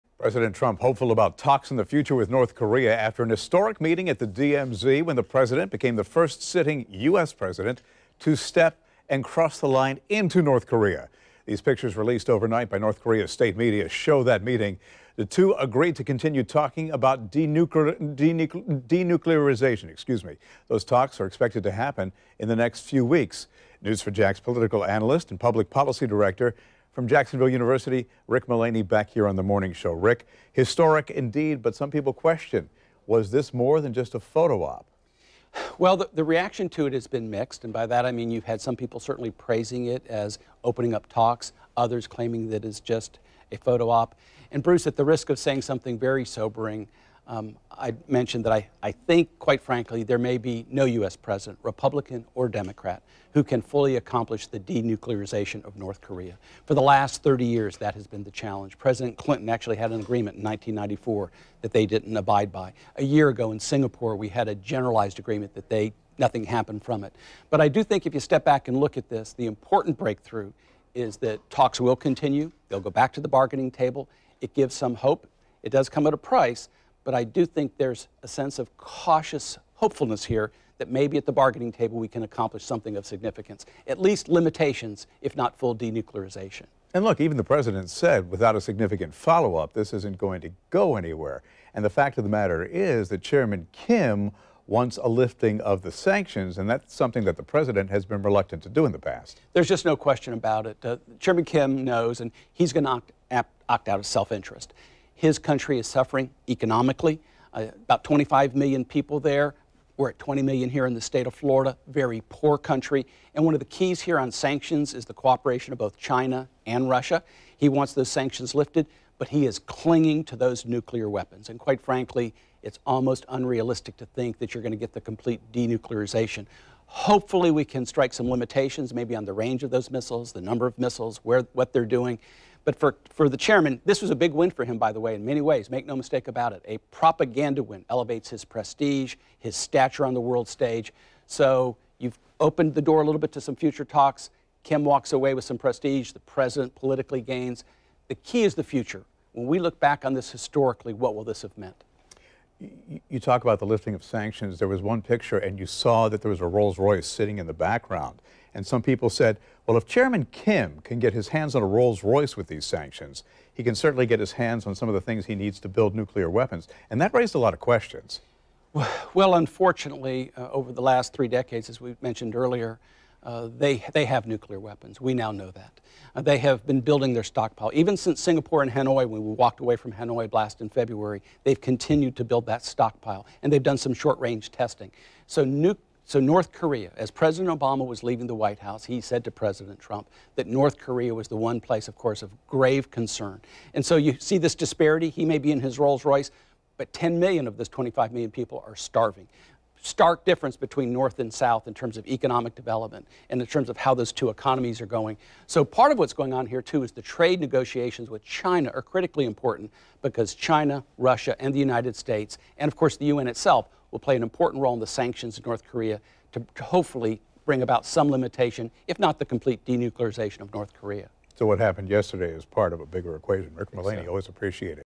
Rick Mullaney reacts to the DMZ and the likelihood of Denuclearization
VIDEO: Bruce sits down with Rick Mullaney to talk about the likelihood of North Korea's denuclearization.